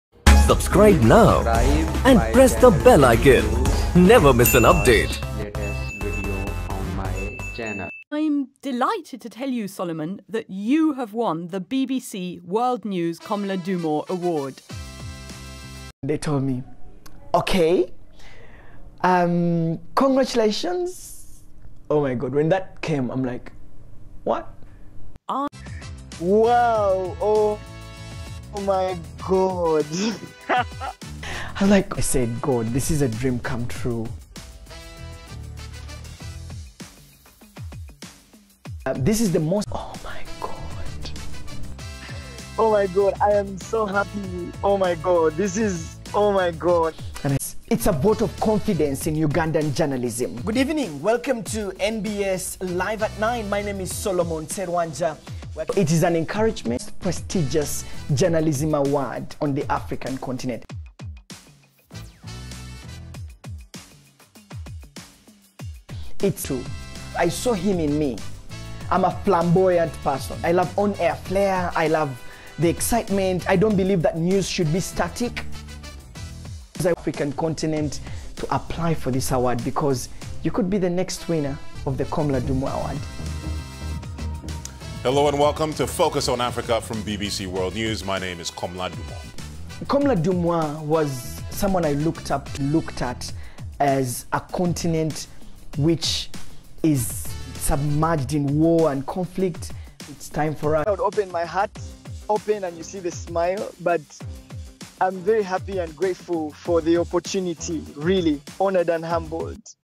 Komla Dumor award 2019: Solomon Serwanjja wins
Komla Dumor award 2019: Solomon Serwanjja wins. Ugandan investigative reporter and news anchor Solomon Serwanjja has won the 2019 BBC World News Komla Dumor Award. Serwanjja is a presenter at Uganda's NBS TV, where he hosts one of the channel's prime-time shows